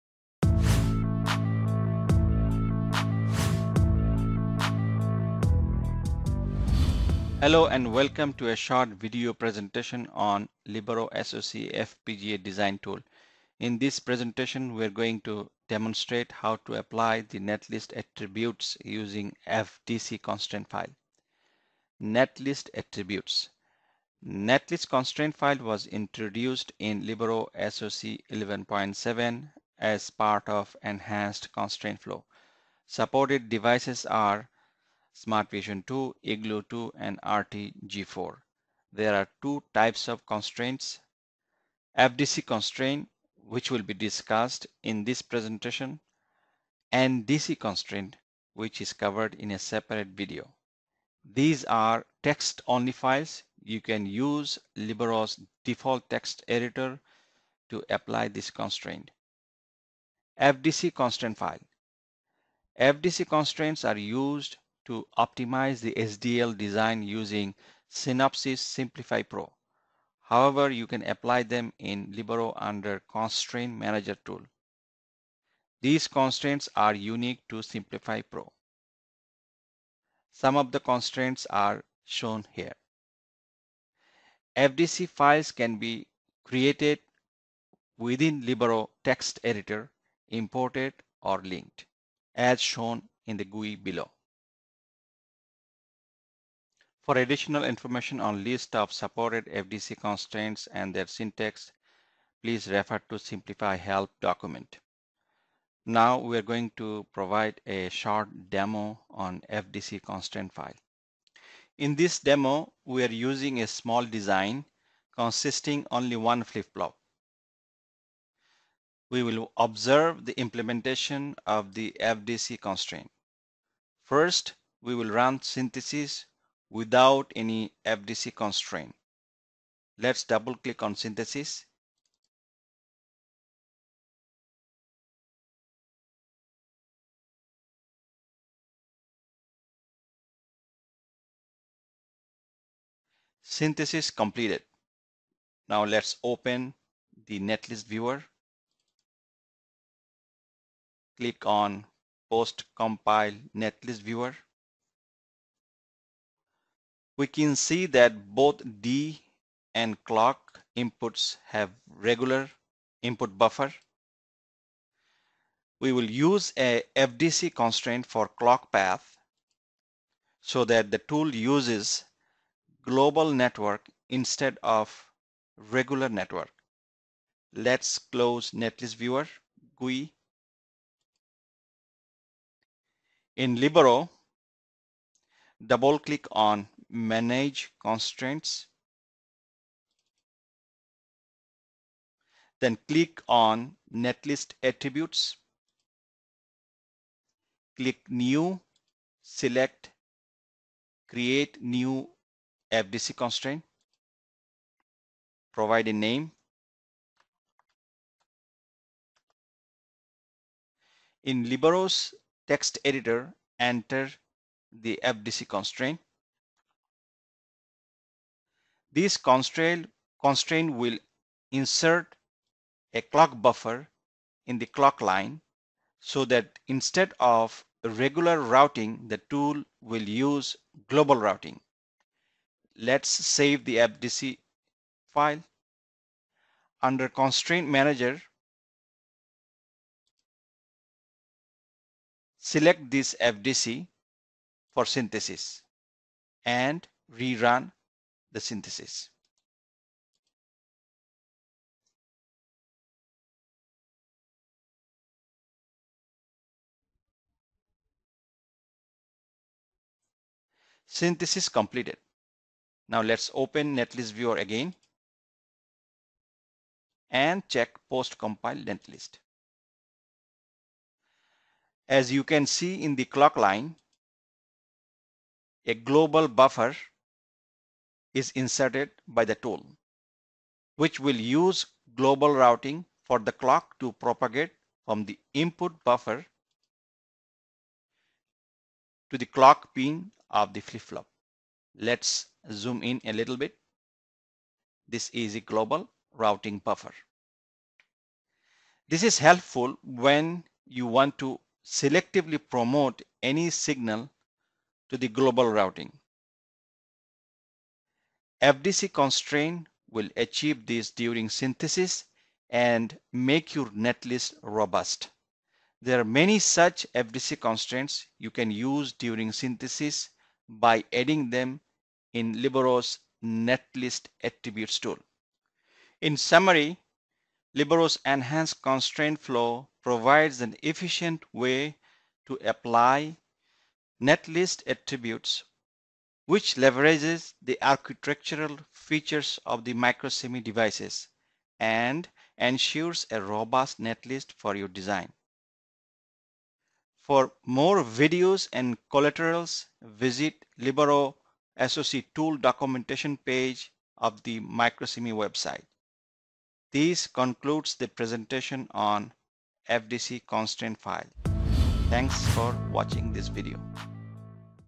Getting started with FPGA Design Constraint (FDC)
This short video will introduce basics of FDC constraint such as syntax, how to apply and a walk-through with Libero SoC tool.